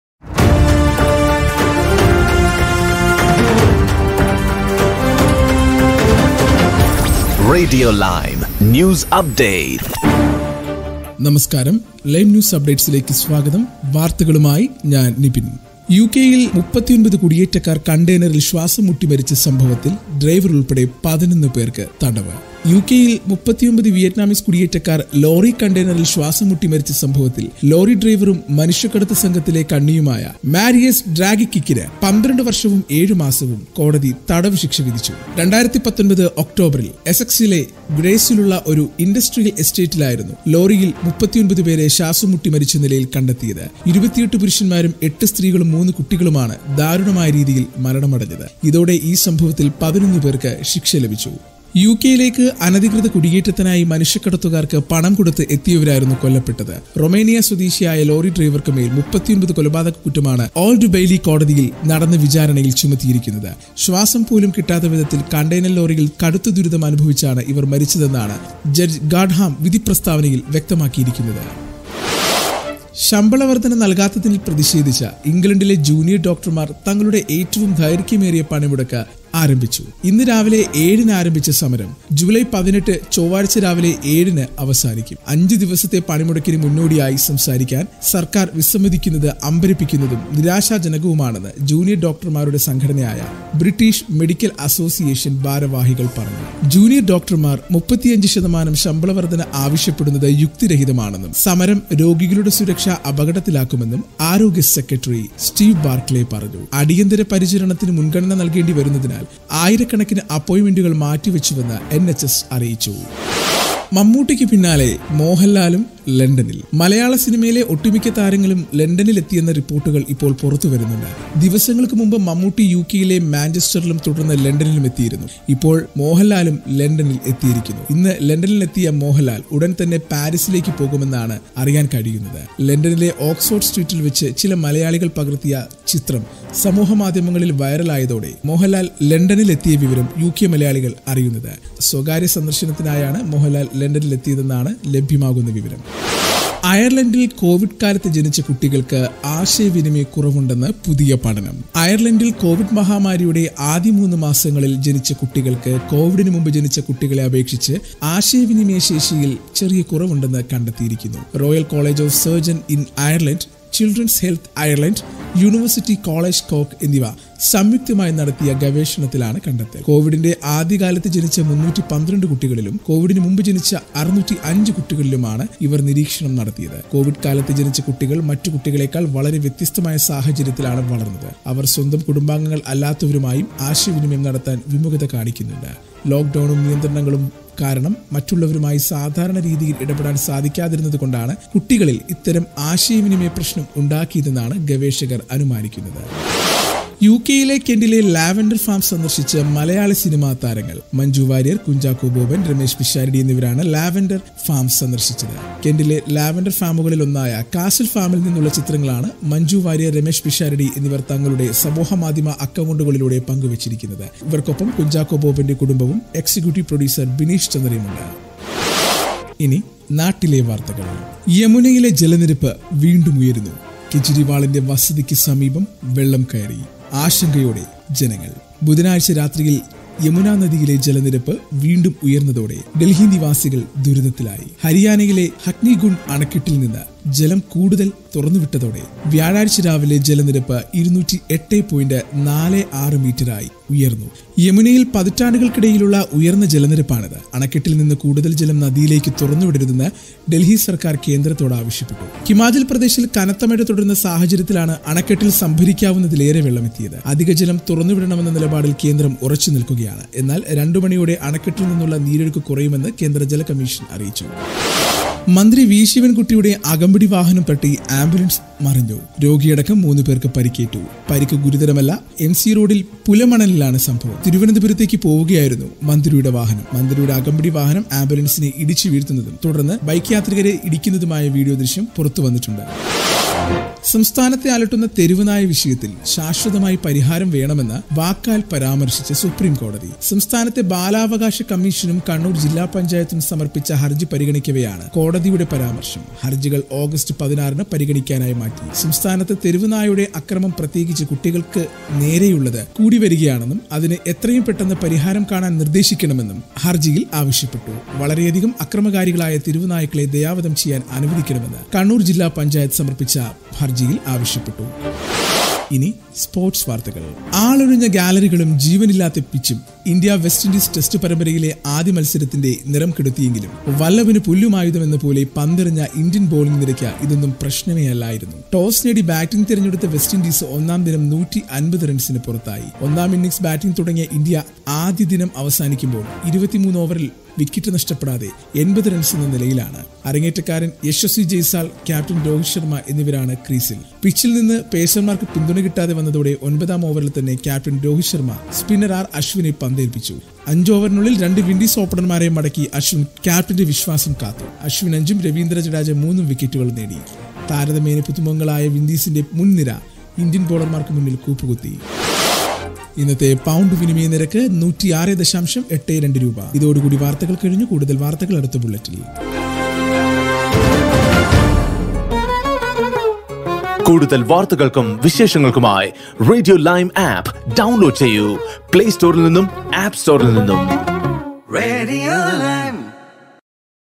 ശമ്പള വർധനവ്; ഇംഗ്ലണ്ടിൽ ജൂനിയർ ഡോക്ടർമാർ പണിമുടക്ക് ആരംഭിച്ചു പണിമുടക്ക് അഞ്ച് ദിവസം|NHS|RJNIBIN|UK
Radio Lime- The First South Indian Multilingual Digital Radio Station in the UK.
How to listen to Radio